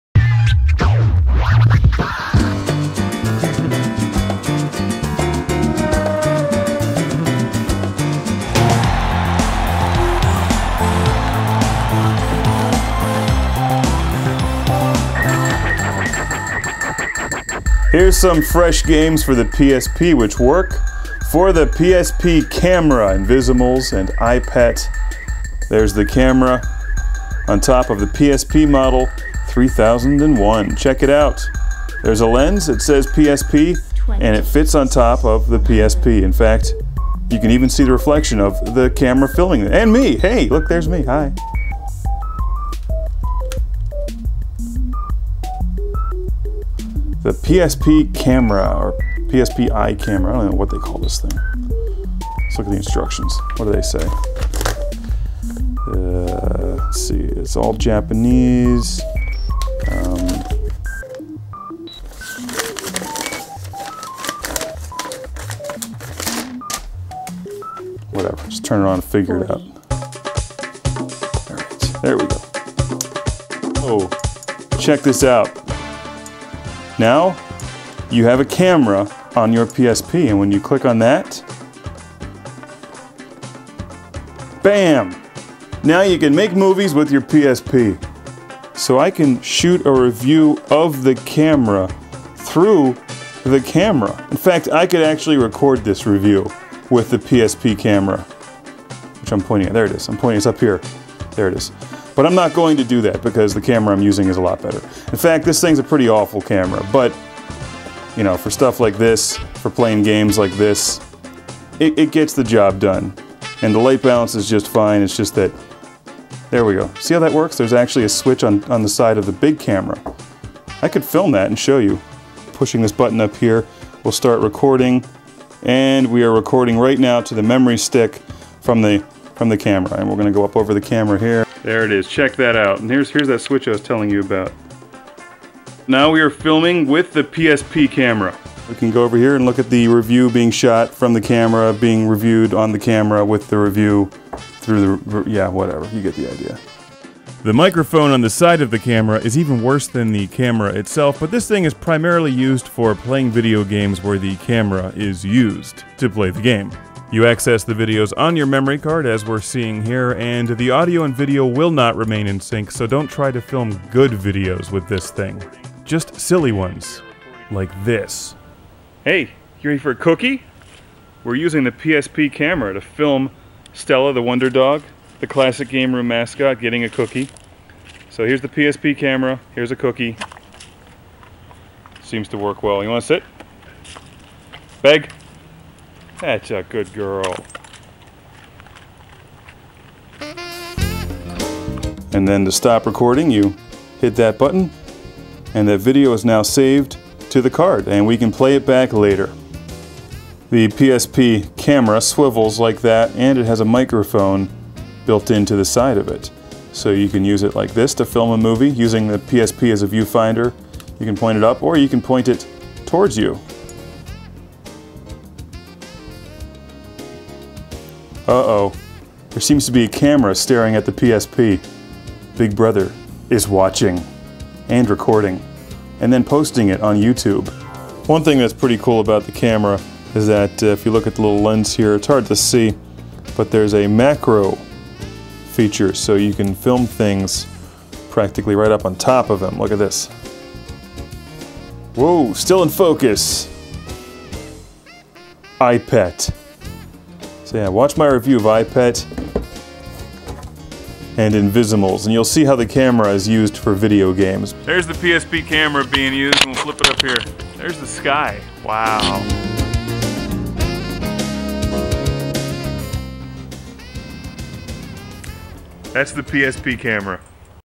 Classic Game Room - PSP CAMERA review
Classic Game Room reviews the PSP CAMERA for use on the Sony PSP handheld video game console. The PSP camera can be used to take pictures, shoot videos and play certain videogames like Eyepet and Invizimals. The PSP camera is not a good camera by any means but it gets the job done, especially when playing games like Invizimals which use the camera effect nicely. The PSP camera has a cheapo microphone and macro function too! From Sony and available on PSP models that aren't the PSP Beaver (aka PSP Go)